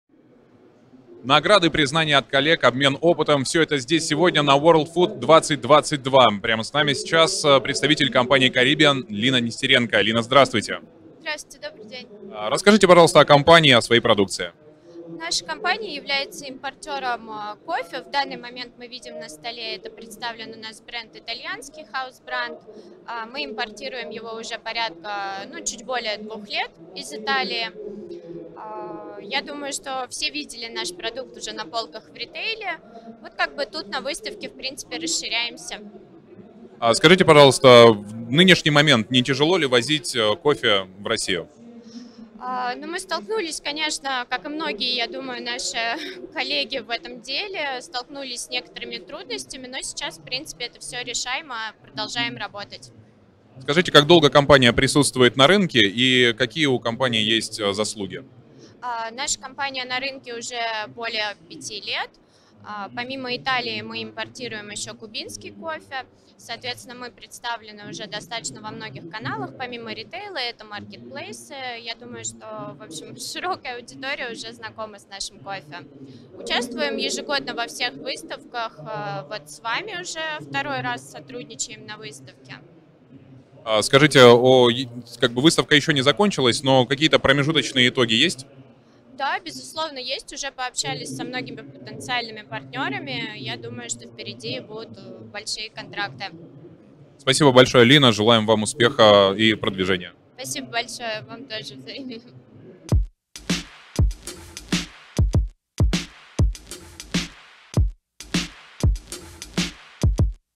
Лина Нестеренко - ООО Карибиан на
Лина Нестеренко, ООО Карибиан, о том, как импортировать итальянский кофе в Россию и каналах сбыта